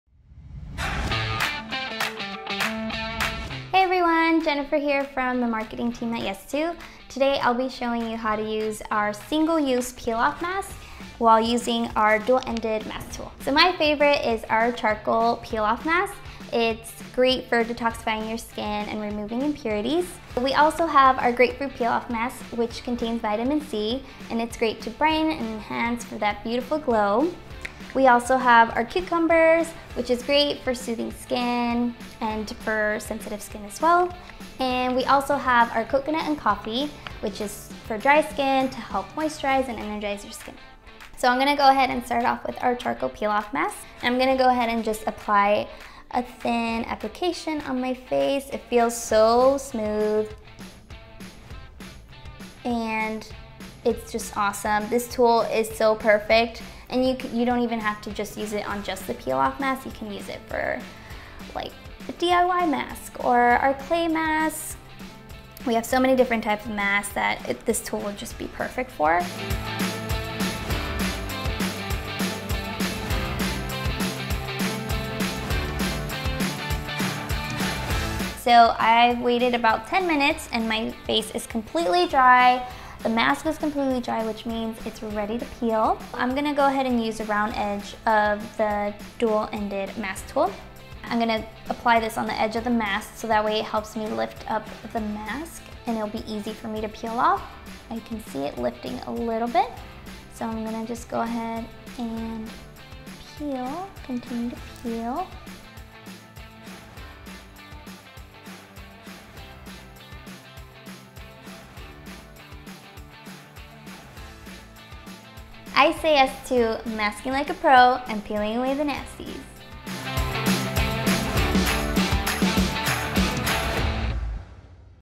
Yes To Peel-Off Masks with 95% Natural Ingredients
Get ready to mask like a PRO! Learn how to use the Yes To Peel-Off Masks to get rid of impurities for clearer, detoxified skin 🖤